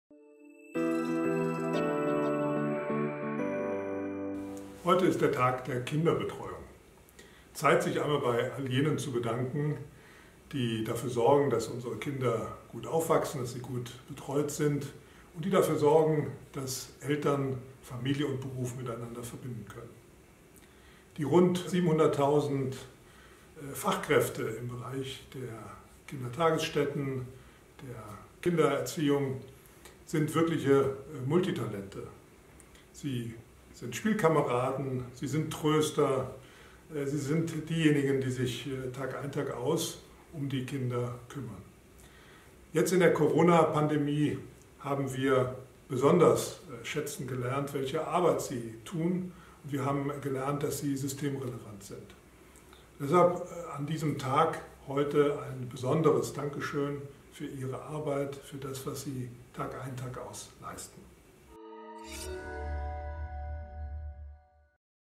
Tag der Kinderbetreuung
Der 10. Mai ist der Tag der Kinderbetreuung. Zeit, sich bei all jenen zu bedanken, die täglich dafür sorgen, dass unsere Kinder bestmöglich aufwachsen und Eltern Beruf und Familie optimal miteinander vereinbaren können. Besonders in der Corona-Pandemie wird den Kita-Fachkräften viel abverlangt – es ist deutlich geworden, dass ihre Arbeit systemrelevant ist.